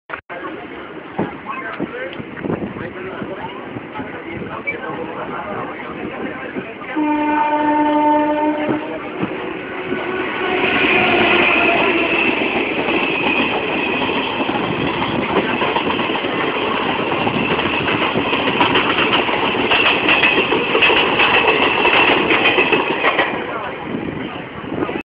ASN BARBIE FLIES OUT OF DDJ WITH INTERCITY..........
19/04/2013:AS OUR BRPA-SDAH EMU CRAWLS TOWARDS DDJ ASN WAG 5 23525 FLIES AWAY WITH 13503UP SDAH-ASN ICE AT ITS TOW.